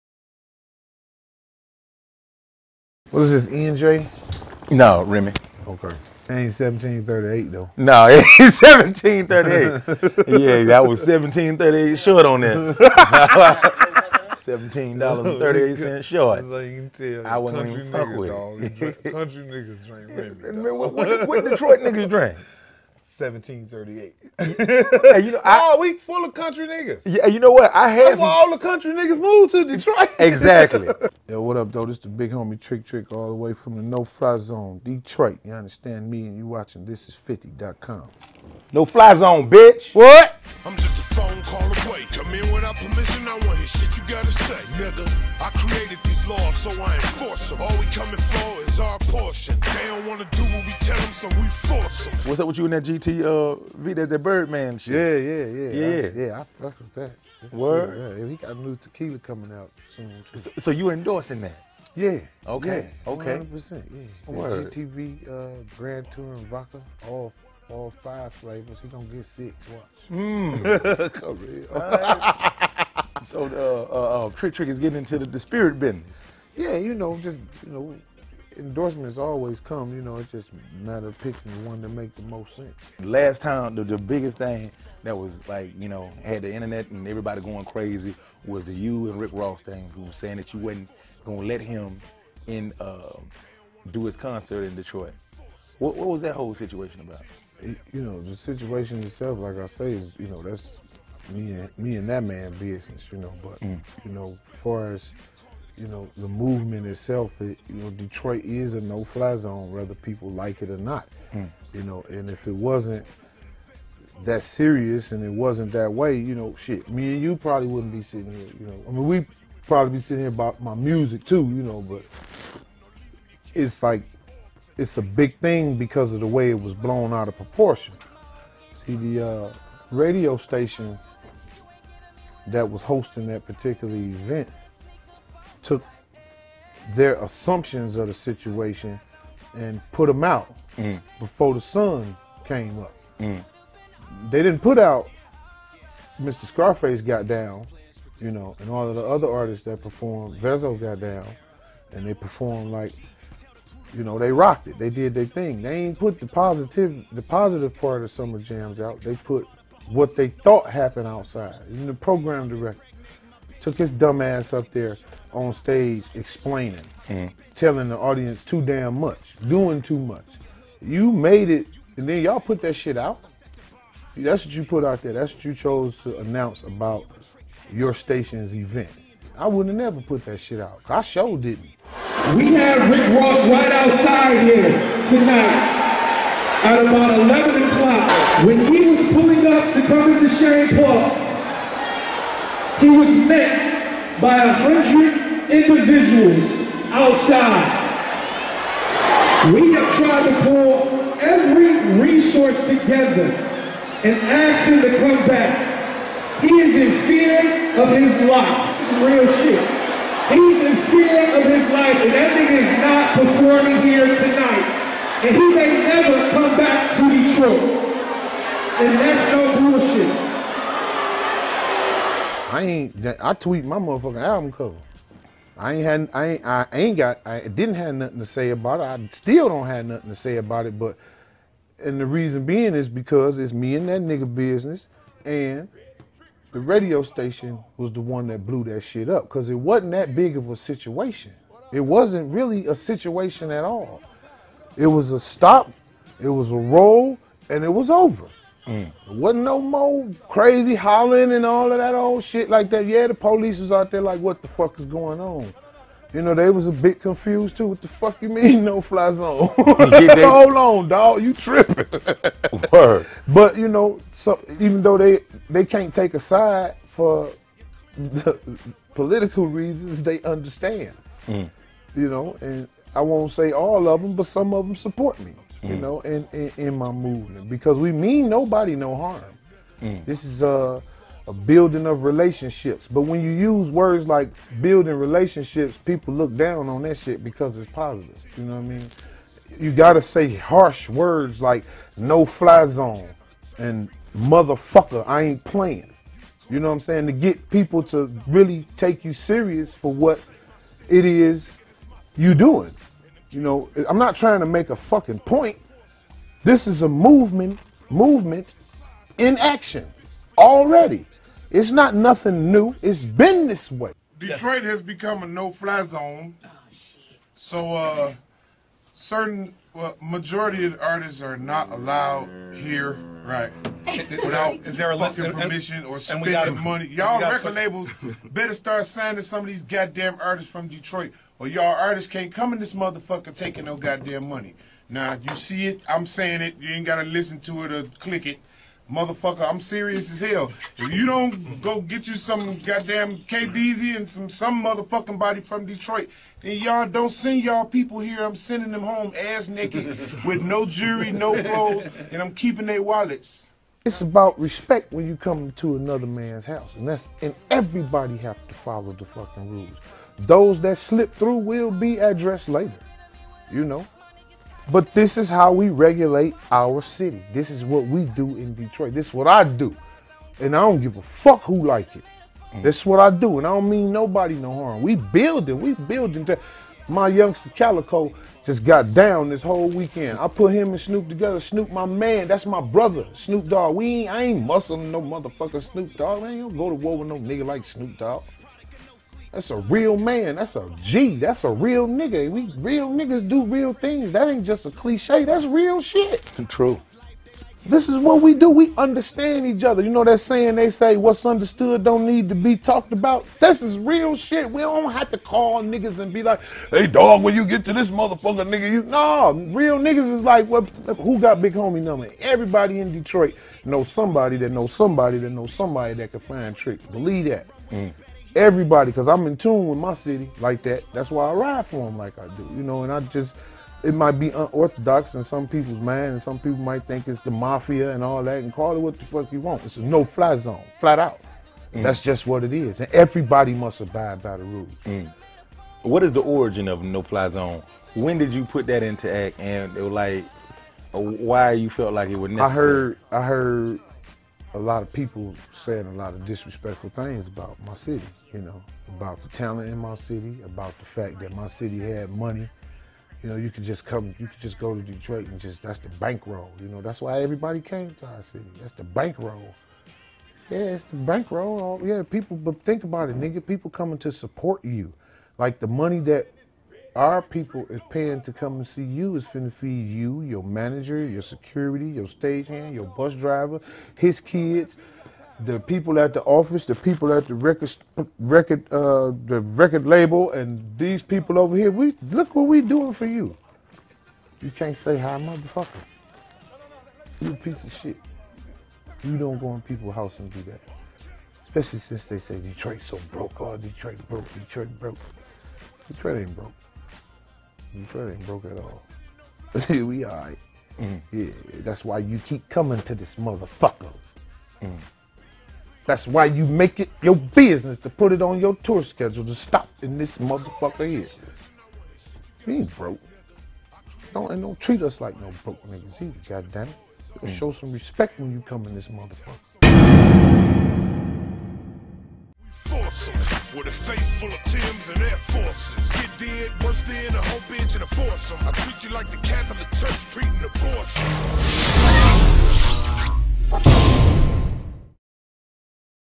Trick Trick Addresses Shutting Rick Ross Out From Performing In Detroit; No Fly Zone
Thisis50 & Young Jack Thriller recently spoke with Trick Trick for an exclusive interview!

Trick Trick addresses shutting Rick Ross out from performing in Detroit, No FlyZone & much more!

Follow @tricktrickgs @jackthriller @thisis50